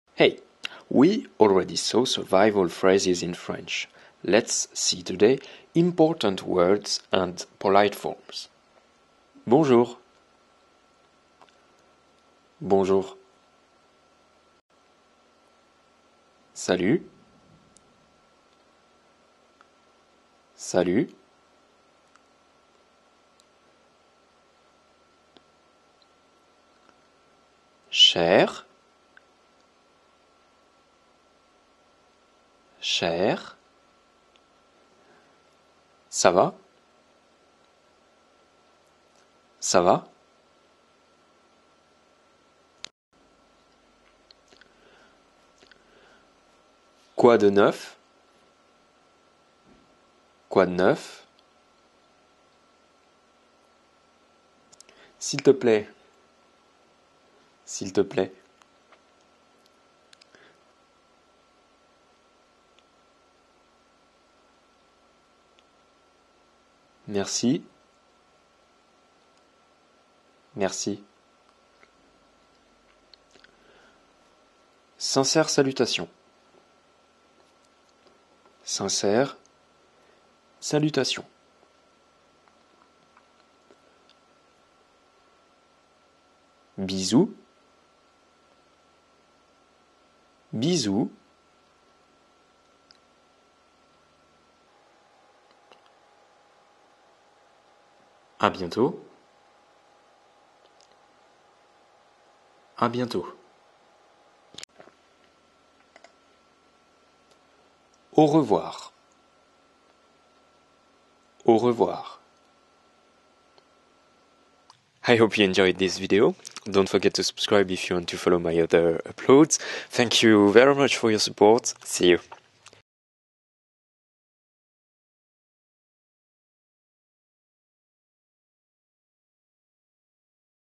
Daily Words and Polite Forms in french [Salut, Bonjour, Bisous]
We already saw, survival kit in French.

Let's discover Greetings in french.

Fast, clear, easy, how to say hi!, how are you?, good bye... in french!